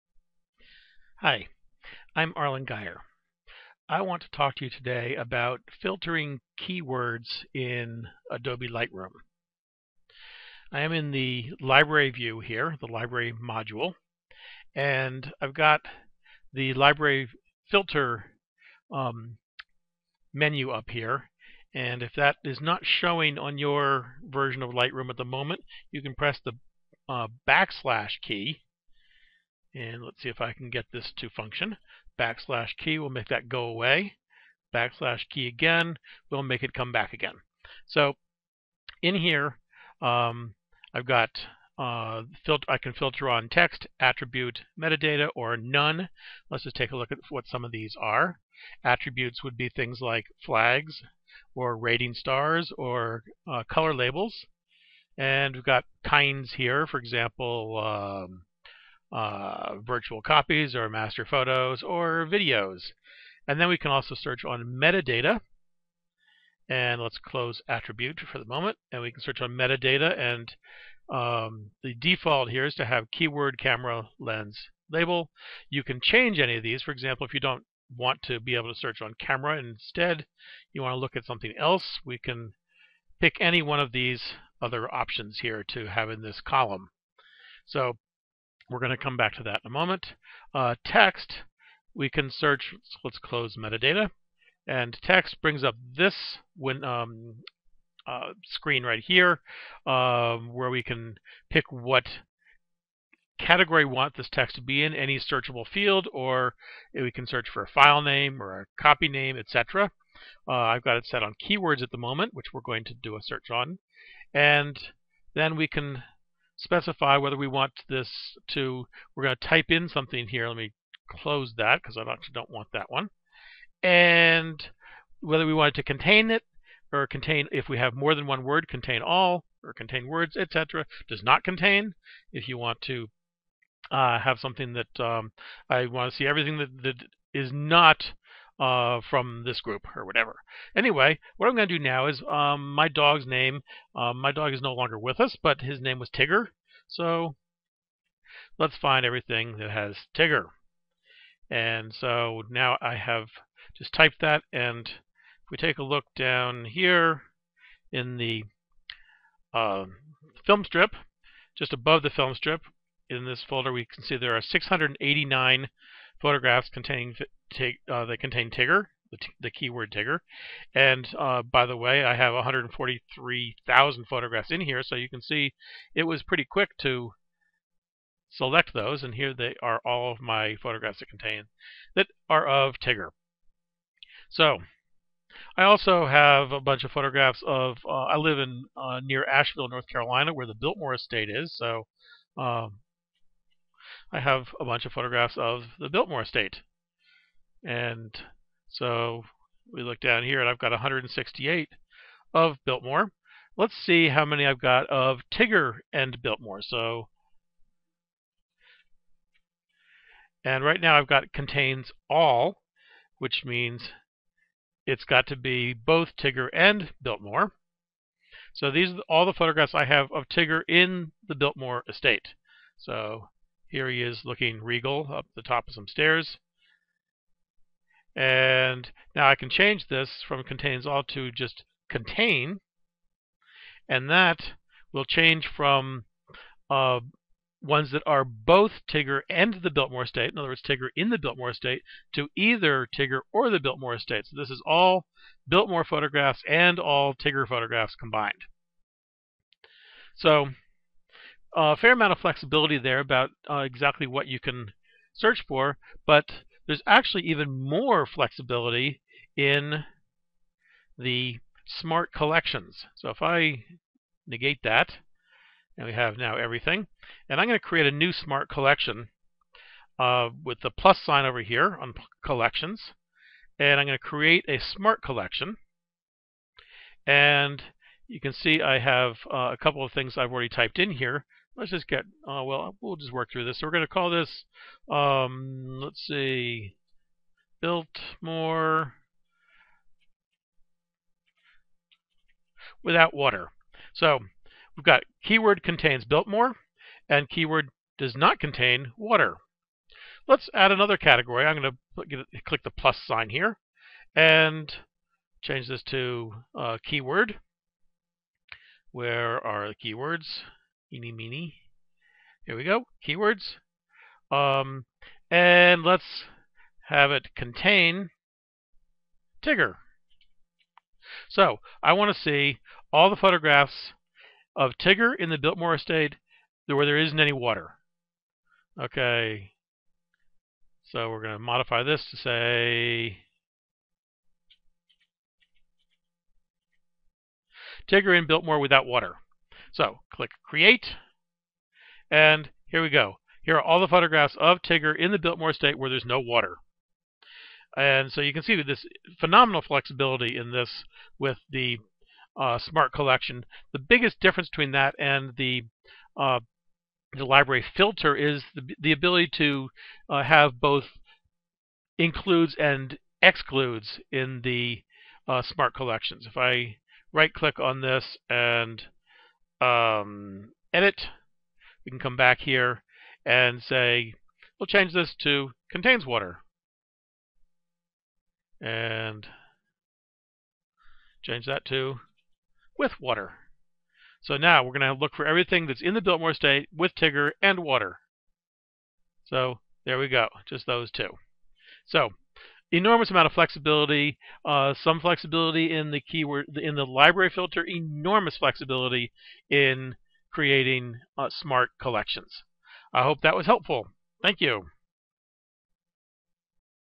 Lightroom - filtering with keywords
How to filter your Lightroom view using keywords, both with the Library Filter and with Smart Collections